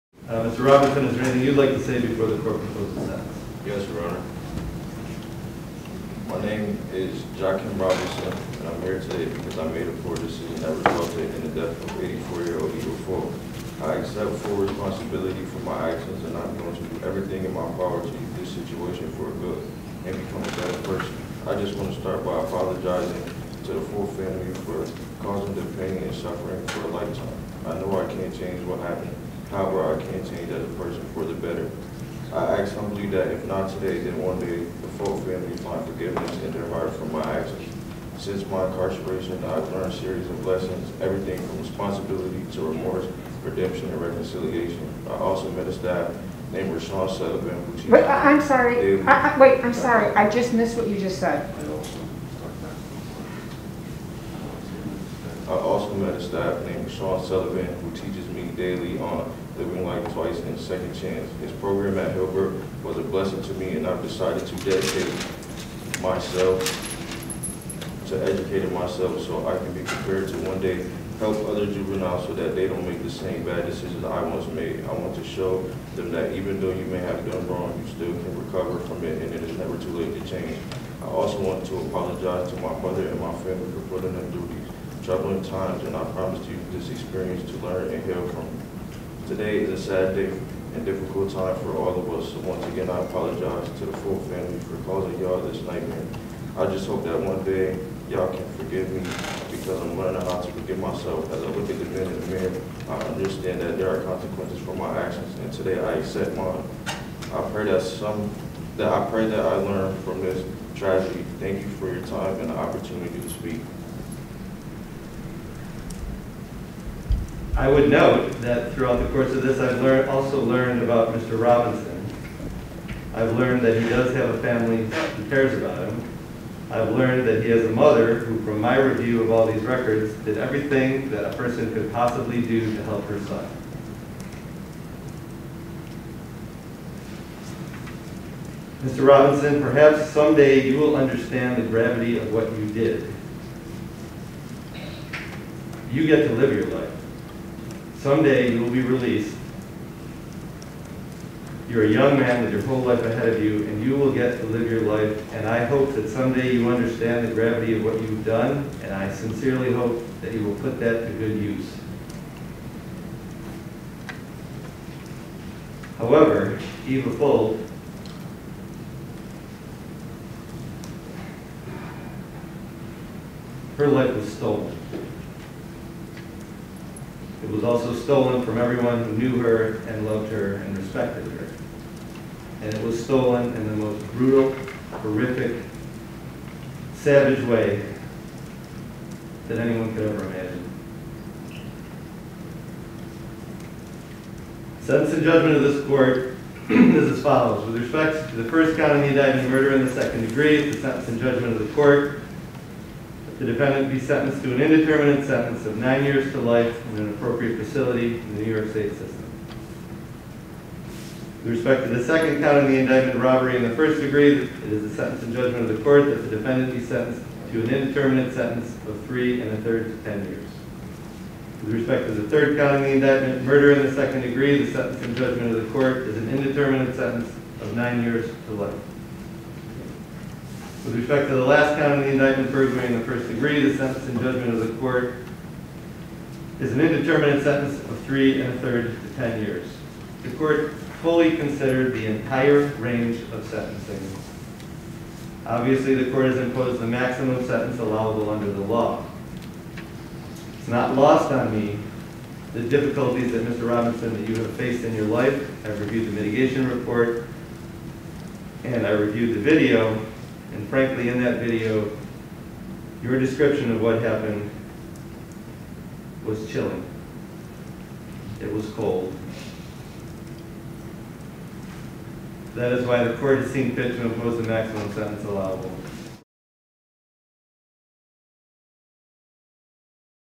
Teen apologizes for grisly murder of 84-year-old Upstate NY woman
During sentencing for the murder of Eva Fuld, a former teacher in Syracuse Thursday, March 10, 2022 at the Onondaga County courthouse, defendant Jahkim Robinson asked for forgiveness from the Fuld family, his family, and the court in which Onondaga County judge Matthew Doran offered his own personal feelings about the details of the case.